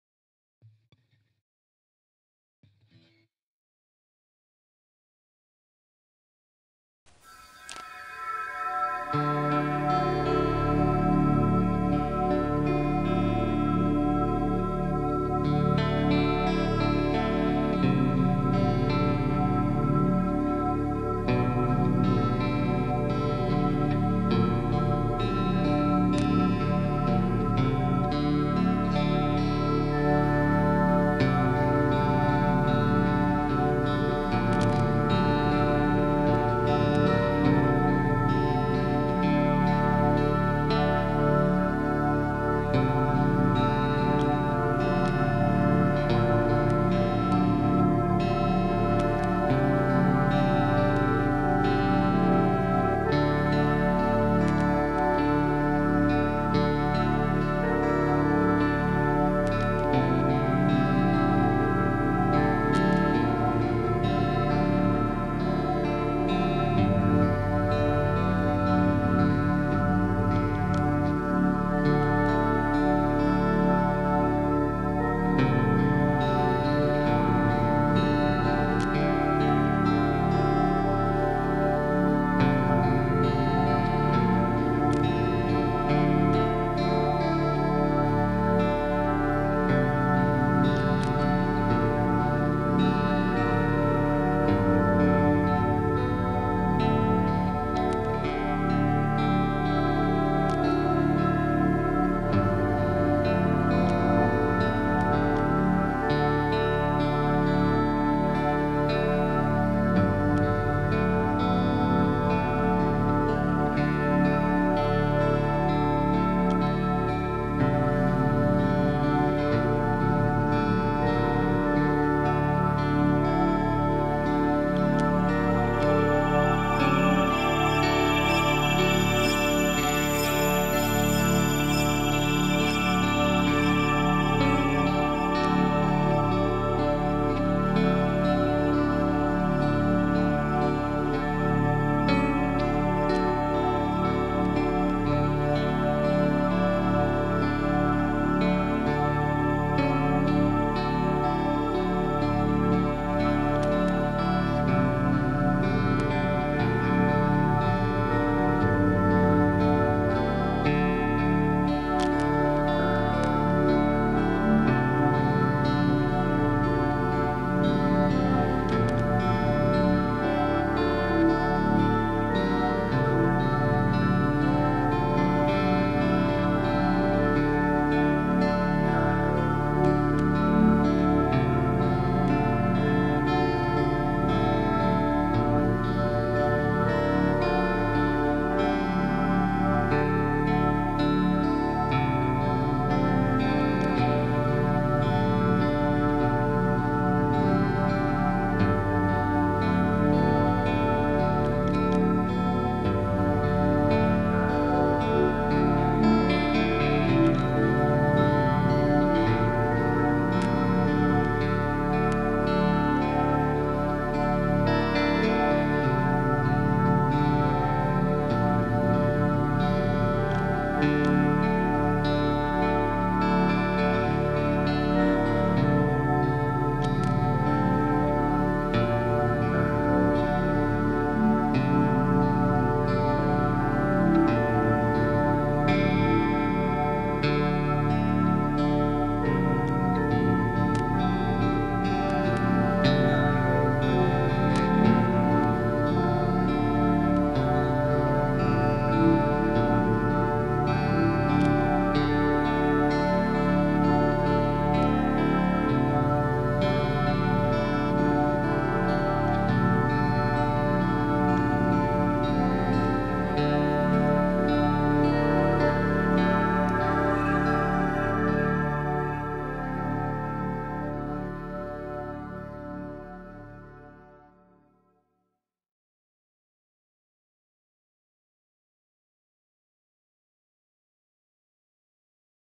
Tai Chi - 2nd Circle with music - Dale Stacy - 2011
Yang Style Long Form . . . 2nd Circle - 40 forms . . . front view & performed simply . . . for previous students to refresh your memories . . . Still as the mountain -- Flowing like water . . . Aloha,